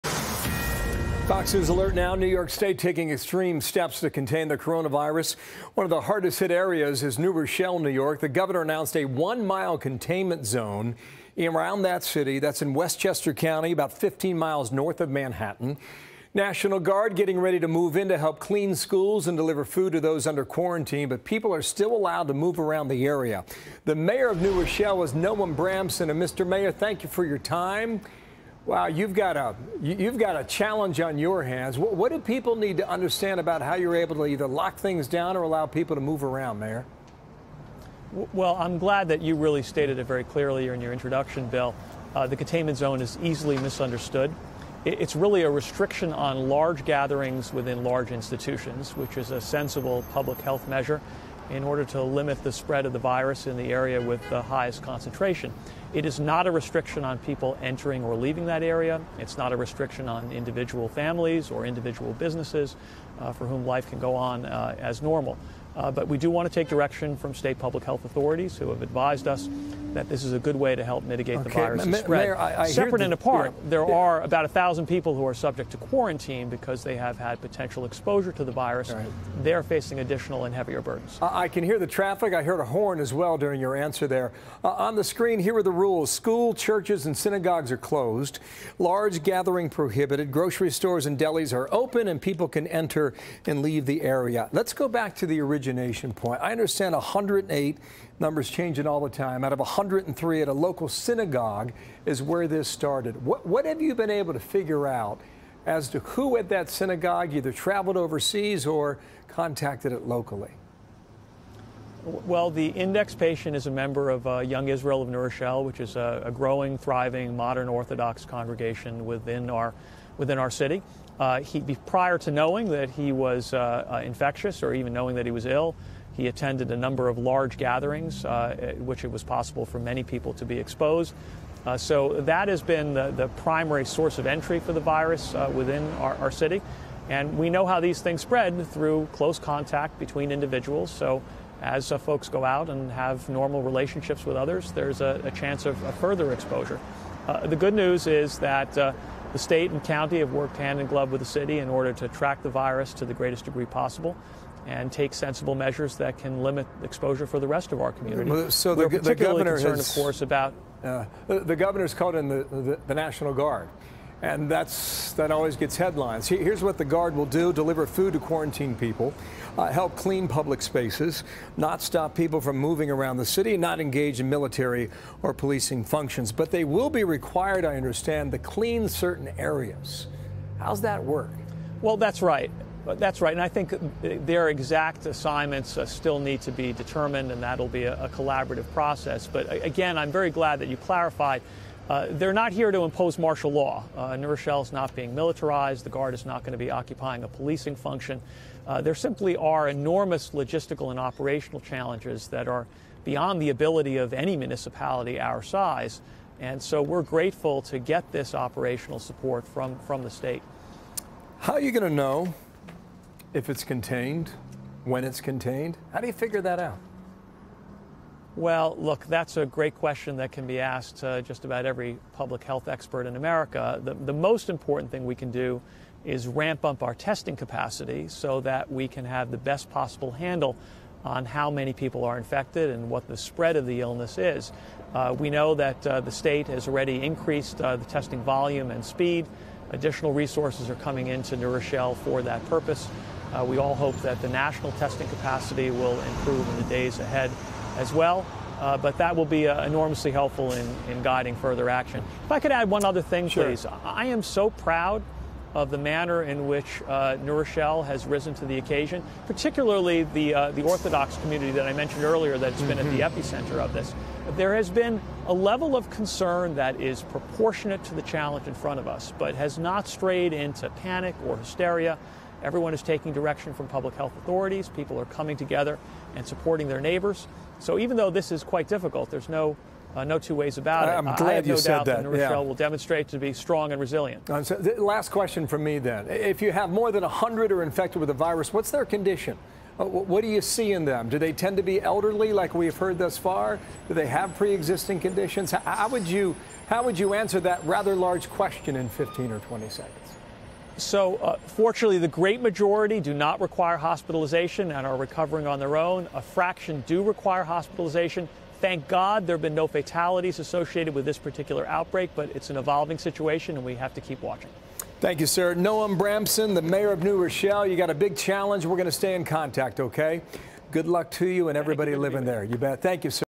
New Rochelle mayor on city's one-mile coronavirus 'containment area'
New Rochelle, New York is not being militarized, says Mayor Noam Bramson.